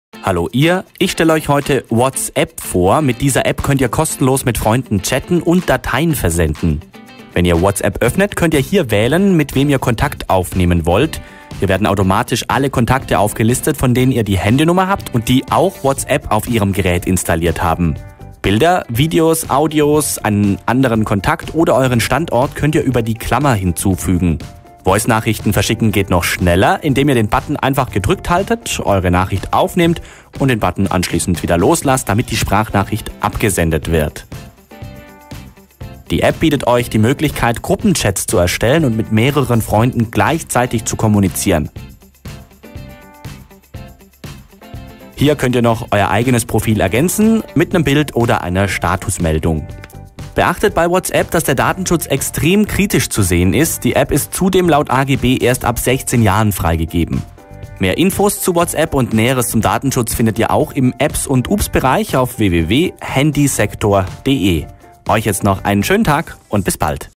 WhatsApp im Handysektor-App-Test
WhatsApp gehört zu den beliebtesten Messenger-Apps und ist damit eine echte Konkurrenz zur SMS. Handysektor hat WhatsApp für euch getestet! Erfahrt hier, wie ihr ganz einfach Nachrichten, Bilder oder Sprachaufnahmen versenden und Gruppenchats starten könnt.